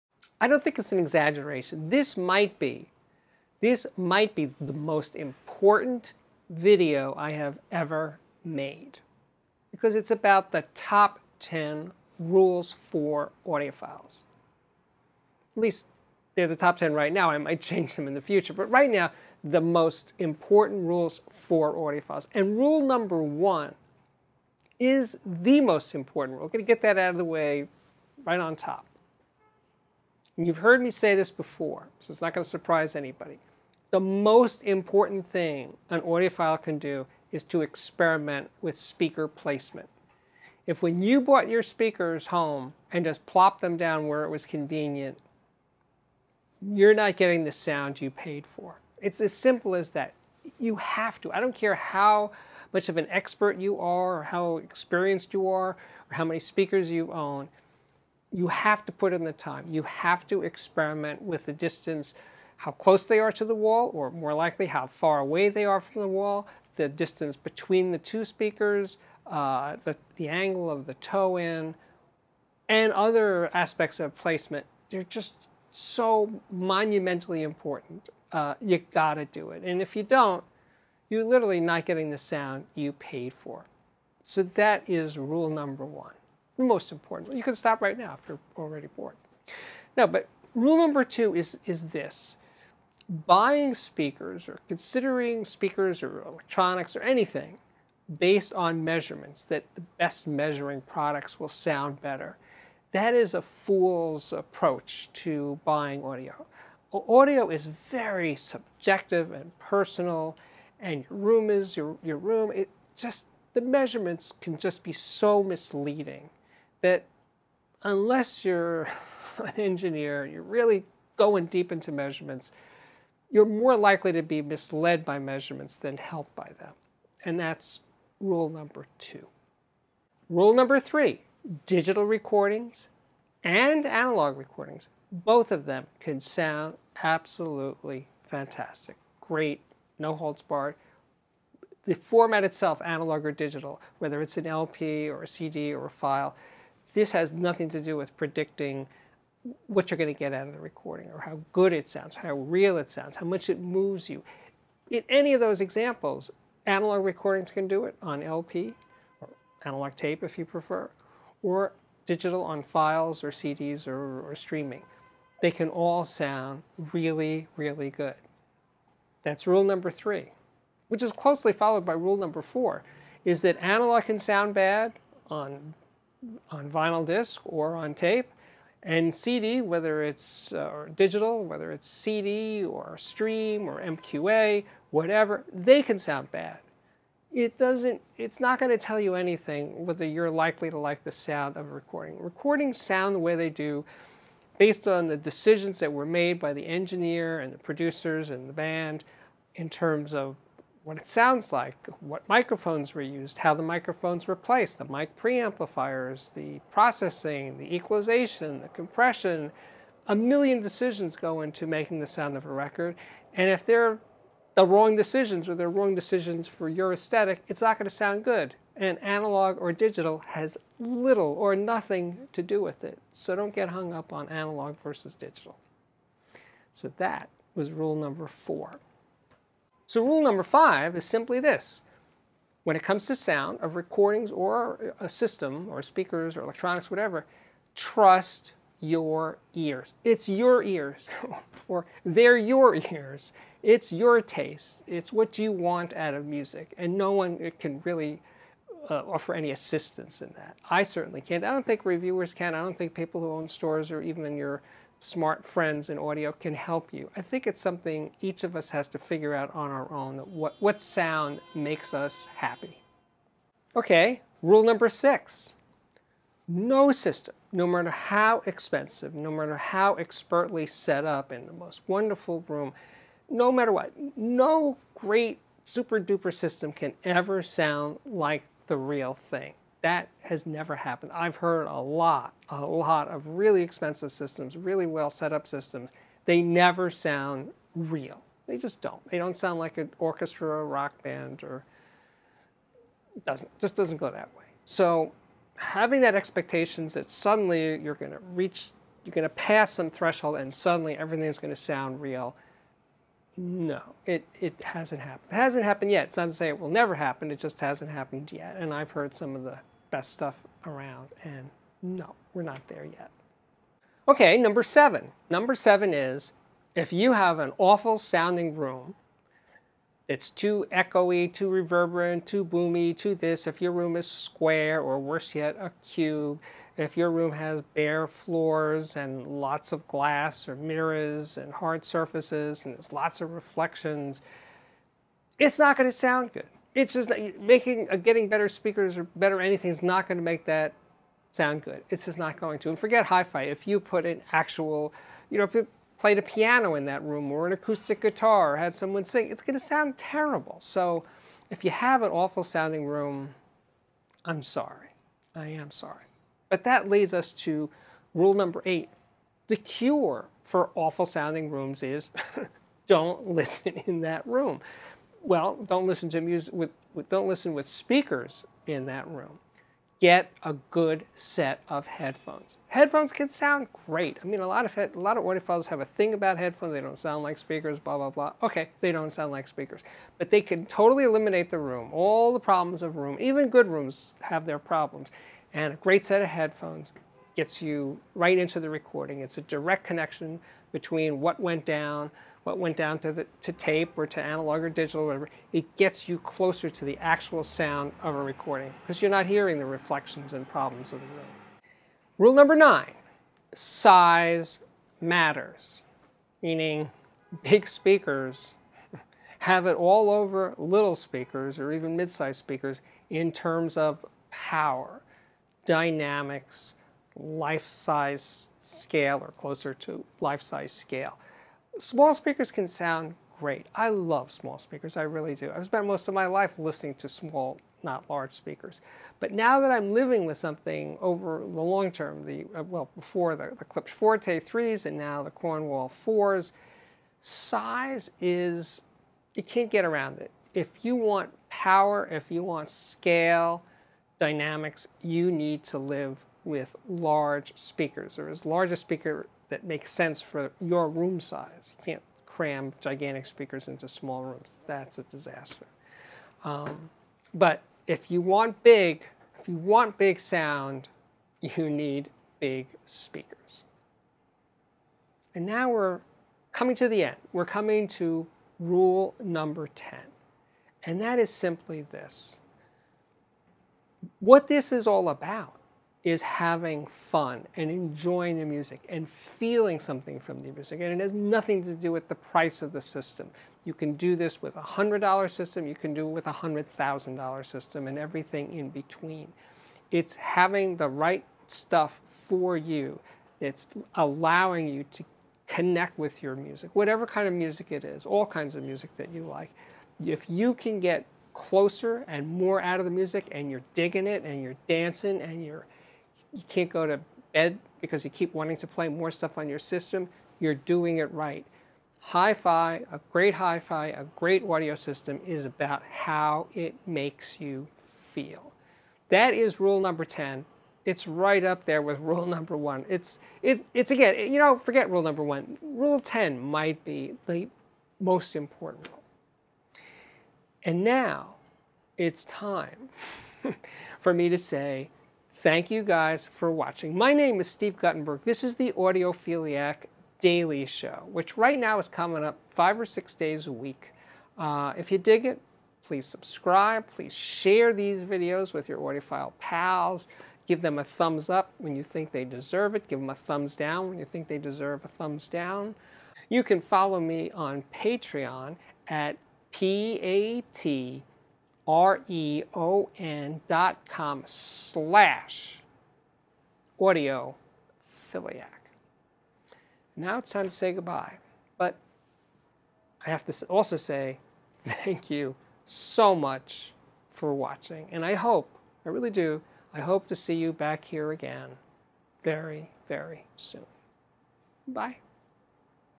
Top 10 rules for audiophiles
The Audiophiliac spells out the absolute best rules and tips for audiophiles.